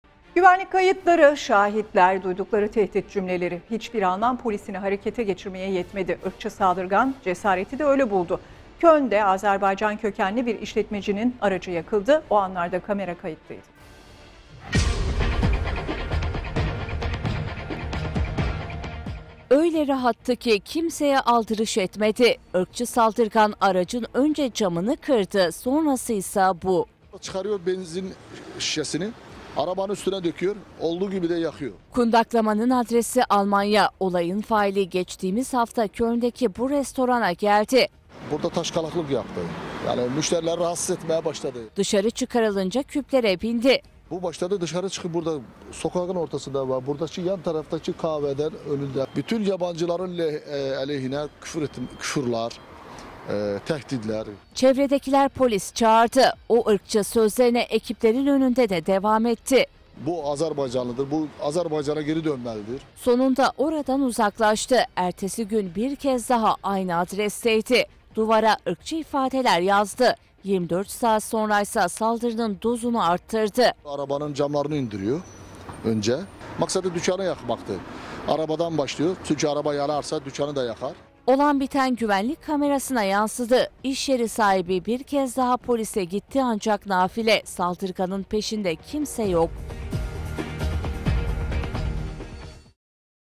Alman Polisi Irkçı Saldırganı Görmezden Geldi
Güvenlik kayıtları, şahitler, duydukları tehdit cümleleri. Hiçbiri Alman polisini harekete geçirmeye yetmedi. Irkçı saldırgan cesareti de öyle buldu. Köln'de Azerbaycan kökenli bir işletmecinin aracını yaktı. O anlarda kamera kayıttaydı.